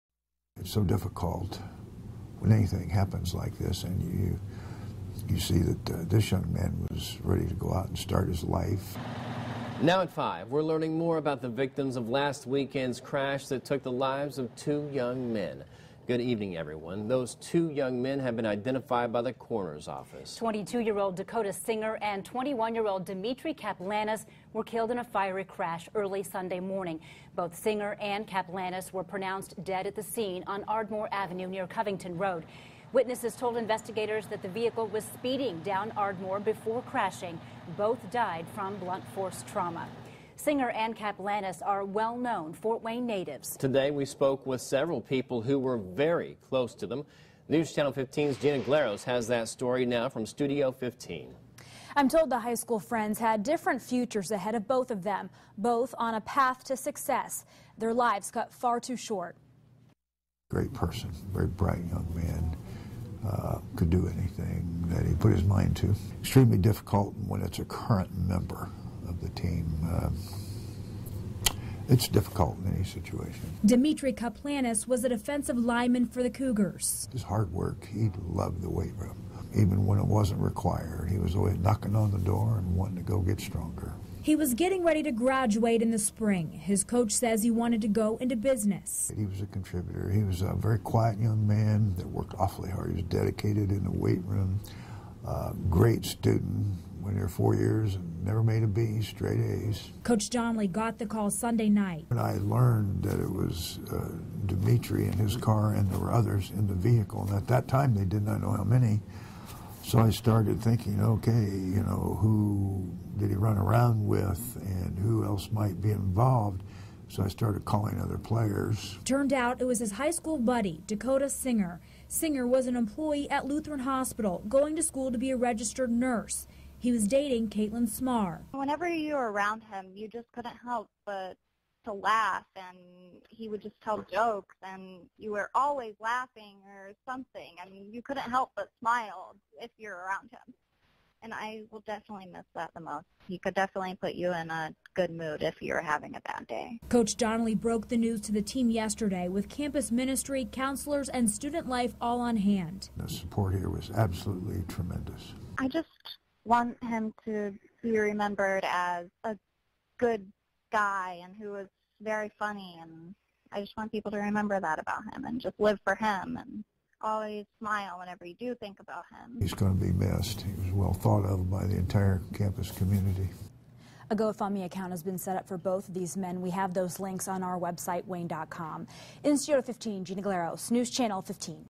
Victims of fiery Ardmore crash ID'd
A community is remembering the two young men killed in last weekend's crash.